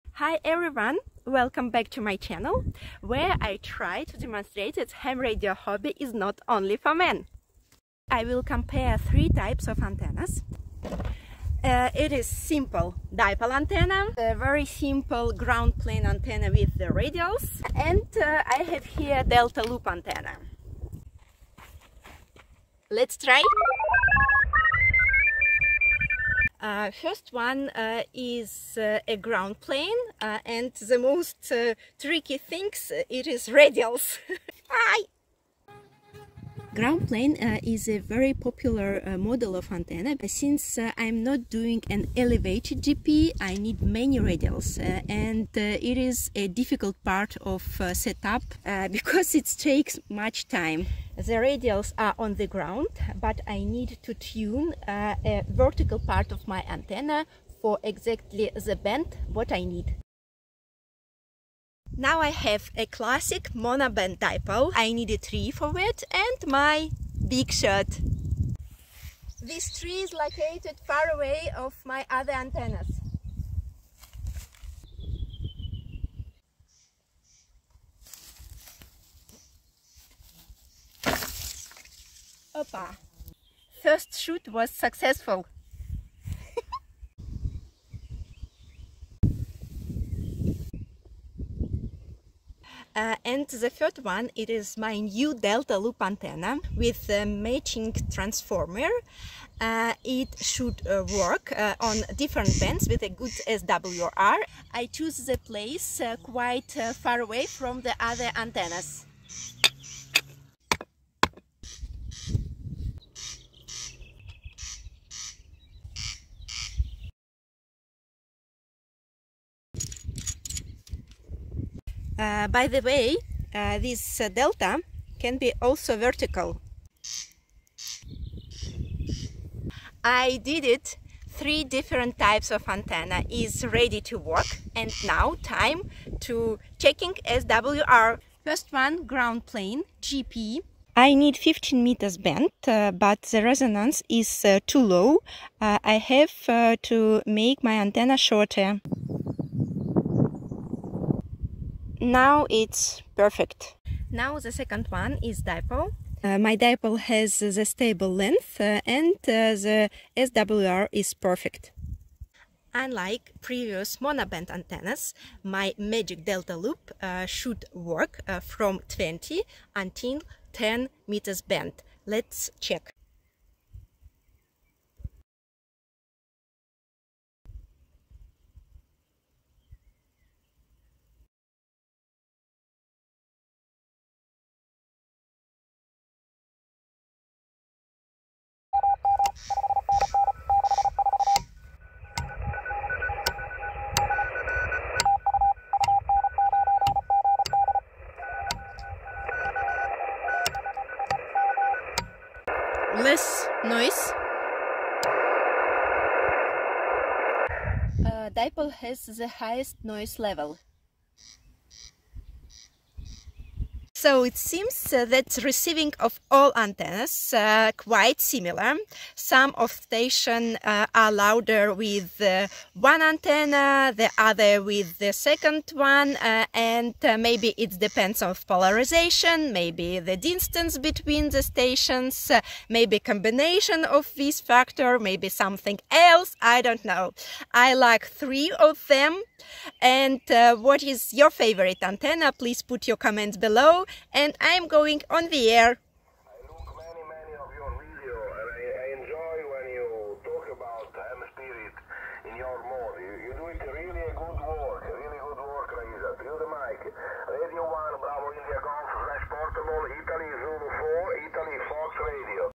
Young Lady's Antenna Challenge: GP, Dipole, Delta - Raisa installs and compares
My comparison doesn't claim to be academic! I wanted to showcase the features of installing, tuning, and RX-operating three different types of portable antennas, and of course, to show that Ham Radio isn't just for men ;-)
What would be your choice?
7388 and see you On the Air!